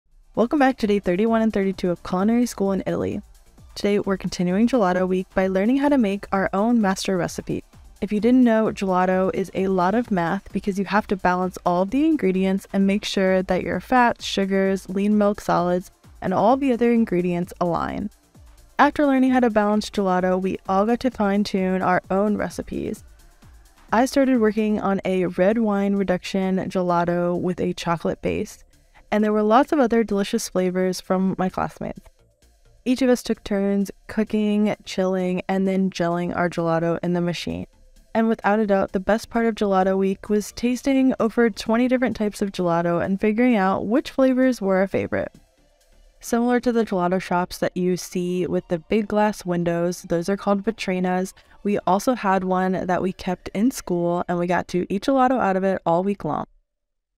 Culinary School In Italy: How To Make Italian Gelato (Day 32 & 33)
Part of going to culinary school in Italy of course includes how to make homemade gelato. Today, we learned how to balance a gelato formula. There's lots of math included in making gelato if you want to have a  delicious final product with a balance of all the ingredients.

Everyone in my class made different gelato flavors, so it was really fun to try them all!